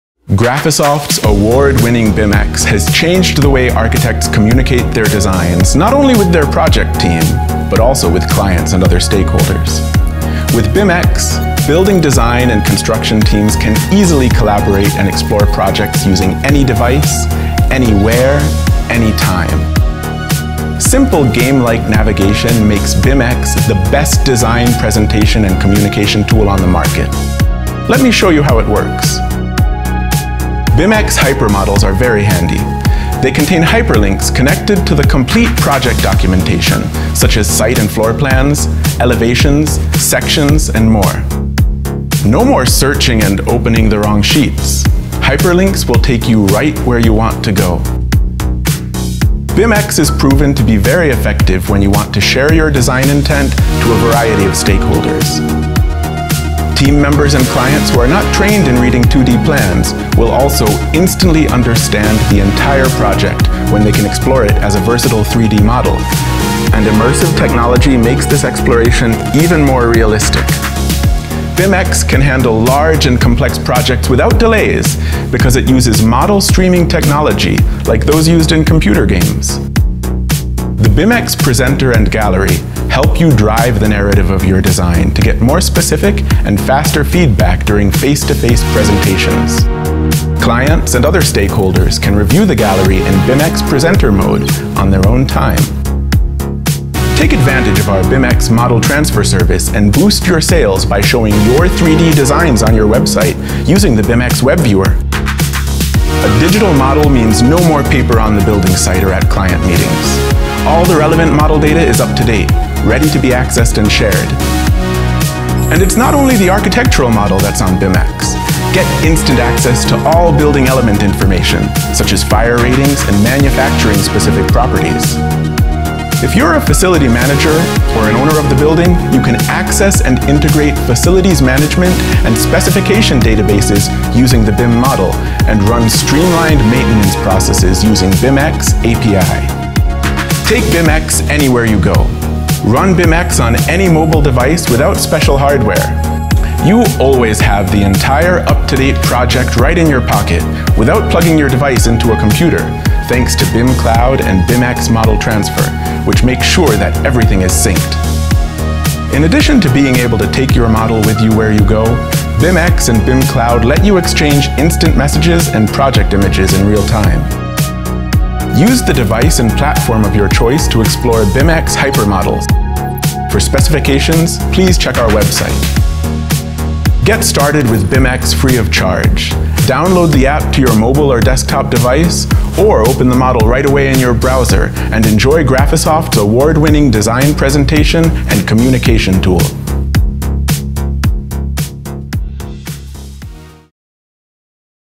Scopri BIMx
BIMx ha cambiato il modo in cui gli architetti comunicano i loro progetti con il team di progetto, i clienti e le altre parti interessate. La ricca documentazione, la navigazione simile a un gioco e le modalità di presentazione ti consentono di offrire un'esperienza di presentazione client senza precedenti. Accelera il feedback e le approvazioni con BIMx.